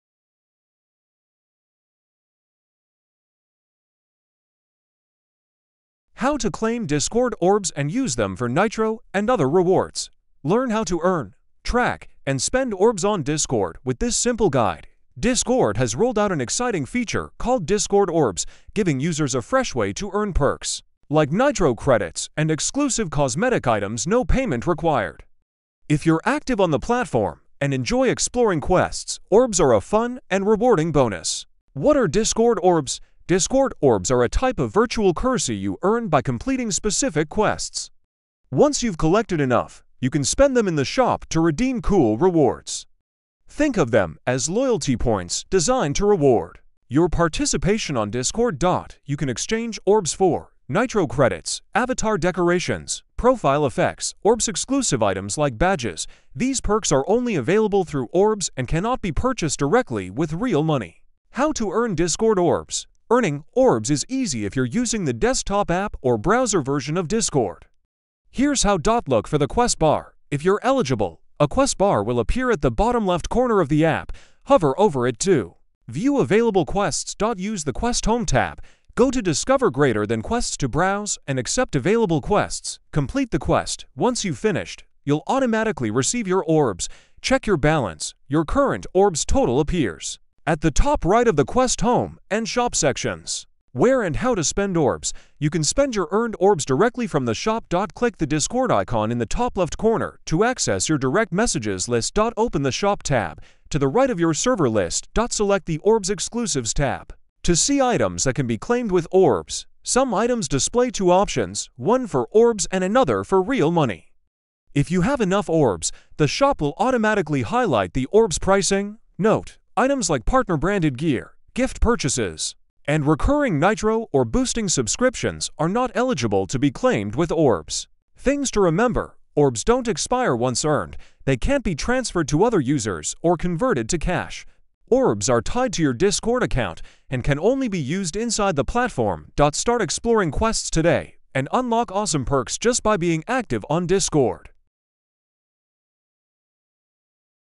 How to Earn Orbs & Redeem 3-Day Nitro on Discord (No Payment Needed)
How to Claim Discord Orbs and Use Them for Free Nitro Rewards! 🎮
Want to know how to earn free Nitro credits and exclusive profile items on Discord? In this video, we’ll walk you through everything you need to know about Discord Orbs—a new rewards system that lets you claim perks just by completing Quests!

📌 What you’ll learn in this video:

What are Discord Orbs and how they work

How to earn Orbs through Quests

Where to find the Quest bar and Quest Home

How to check your Orbs balance

How to spend Orbs in the Shop for Nitro credits, profile effects, and more

Which items are exclusive to Orbs and which ones are not eligible

💡 No credit card or payment needed—Discord Orbs are 100% free to earn and use!

If you're using Discord on desktop or browser, you’ll love this easy guide to make the most of your account and unlock awesome new features. Whether you're after Nitro perks or cool cosmetic upgrades, Orbs are your ticket to free rewards.